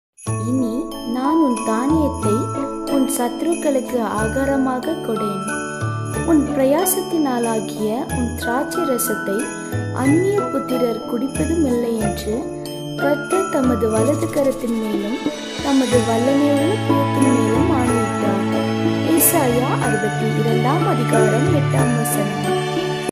Daily Manna#Words of God#பைபிள் வசனங்கள்Tamil Bible verse#Today's verse#verses for whatsapp status
இனி நான் உன் தானியத்தை உன் சத்துருக்களுக்கு ஆகாரமாகக்கொடேன்; உன் பிரயாசத்தினாலாகிய உன் திராட்சரசத்தை அந்நிய புத்திரர் குடிப்பதுமில்லையென்று கர்த்தர் தமது வலதுகரத்தின்மேலும் தமது வல்லமையுள்ள புயத்தின்மேலும் ஆணையிட்டார்.

ஏசாயா 62:8